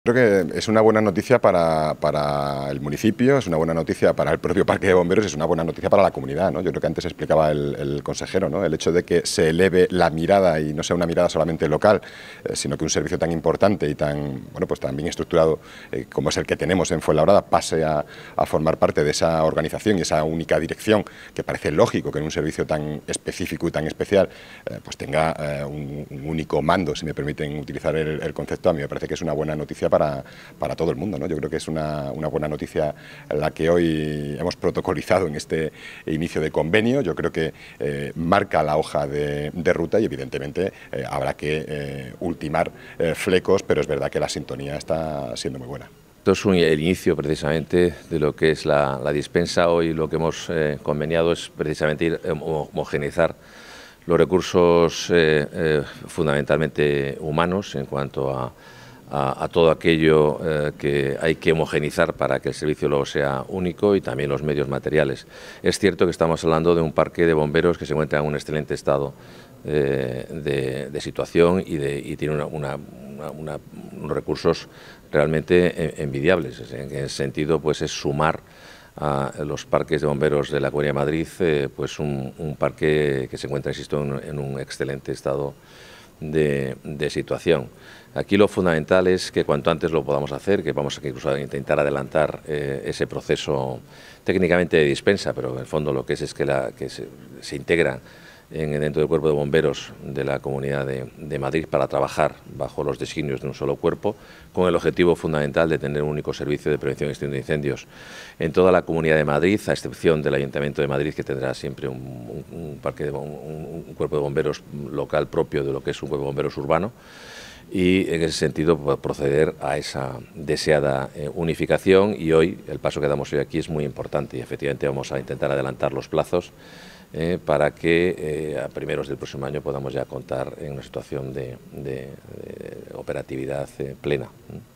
Firmado un acuerdo que regula el proceso de traspaso del parque de bomberos a la Comunidad
El alcalde Javier Ayala y el consejero de Presidencia Enrique López han firmado hoy un convenio que regula el proceso de traspaso del Parque de Bomberos de Fuenlabrada, de titularidad municipal a la Comunidad de Madrid, previsto a lo largo del próximo año.

Dicho convenio establece el proceso a realizar hasta la firma del acuerdo definitivo que establecerá el traspaso. Especifica los cauces de colaboración necesarios que permitirán una adecuada integración del servicio municipal de extinción de incendios como el intercambio de información sobre medios humanos y materiales, así como acciones formativas para la plantilla.

El Ayuntamiento de Fuenlabrada y la Comunidad llevan mucho tiempo trabajando en este traspaso. El pasado mes de febrero, el pleno aprobó el inicio del proceso para la integración del Cuerpo de Bomberos de la ciudad en la red de la Comunidad de Madrid.

La integración que cuenta con el acuerdo de la plantilla no supondrá cambio alguno en la configuración del parque y mejorará la prestación del servicio al sumarse el Cuerpo a la red de emergencias de la Comunidad de Madrid. Además, no supondrá un coste adicional para el Ayuntamiento de Fuenlabrada.

Según las previsiones tanto del Ayuntamiento como de la Comunidad, el traspaso debería producirse antes del 1 de enero de 2024.